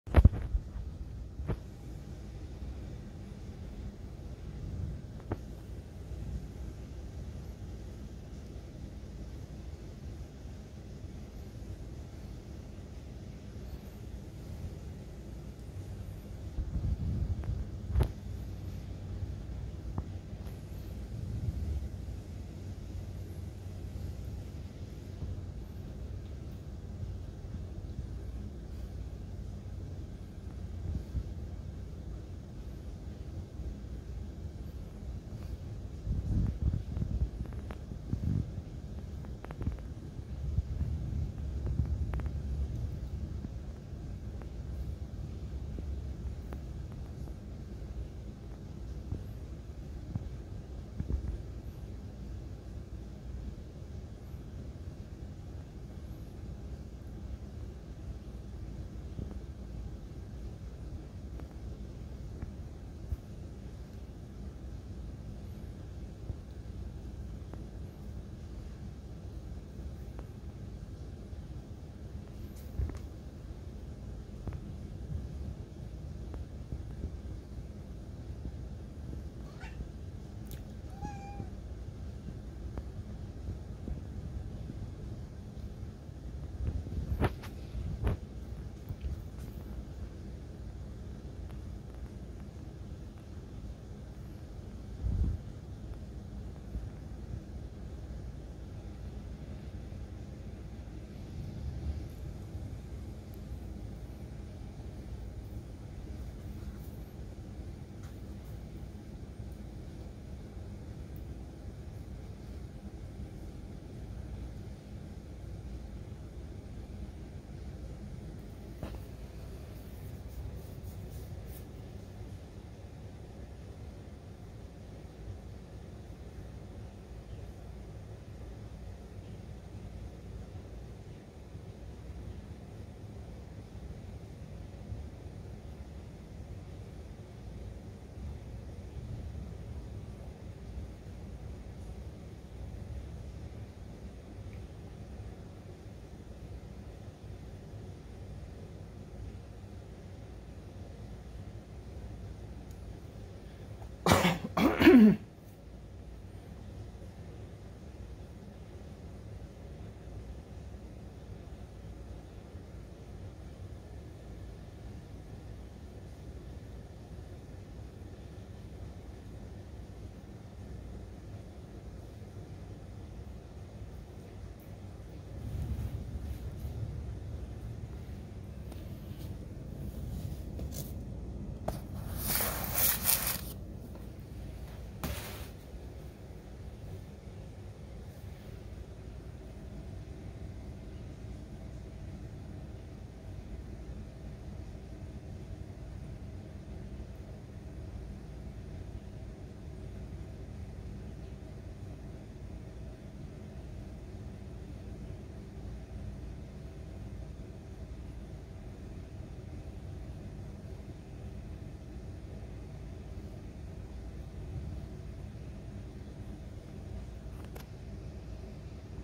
Chloe murders a roach
Chloe the cat killing one of the thousands of roaches in our disgusting apartment building that our Texas landlord said definitely wasn't a problem. July 19th 2021